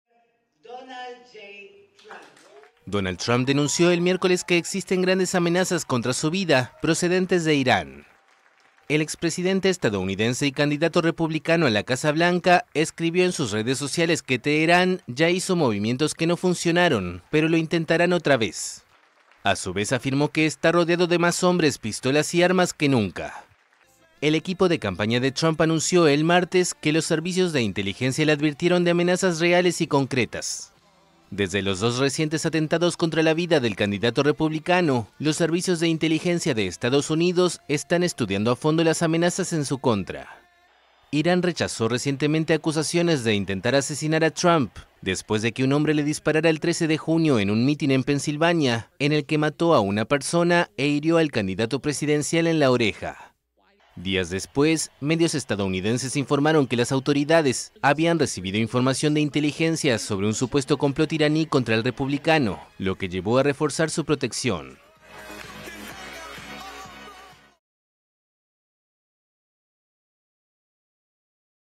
Trump dice que hay "grandes amenazas" contra su vida "desde Irán"
Donald Trump dijo el miércoles que existen "grandes amenazas" contra su vida procedentes de Irán, después de que el equipo de campaña del candidato republicano a las presidenciales de Estados Unidos anunciara que los servicios de inteligencia le advirtieron de amenazas "reales y concretas".